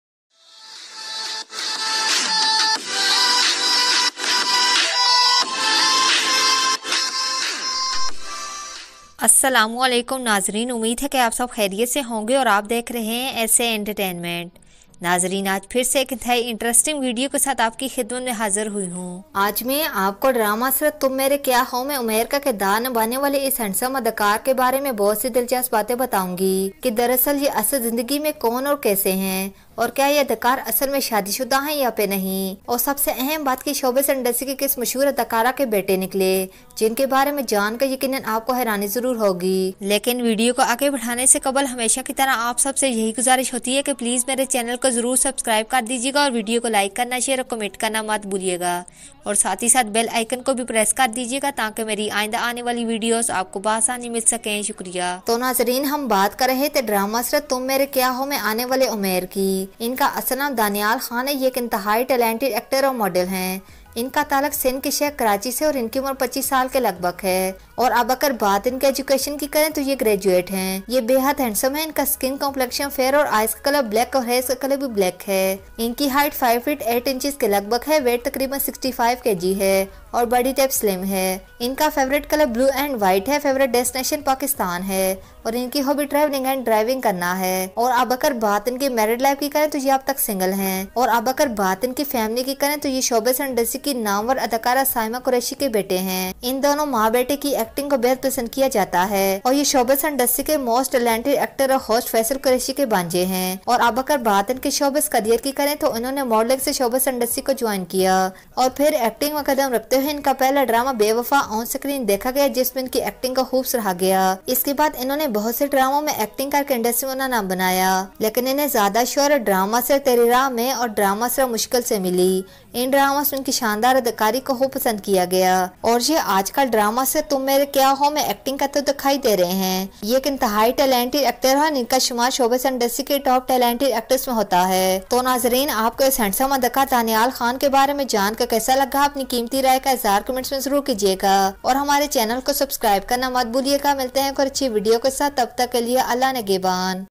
Tum Mere Kya Ho Drama Actor Umair Real Name Family |Tum Mere Kya Ho Last Episode 84
Asalamualikum
Daniyal khan one of the most famous pakistani actor and model. Daniyal khan new drama tum mere kya ho play role umair outstanding acting in drama serial. Daniyal khan son of famous pakistani actress. Daniyal khan complete biography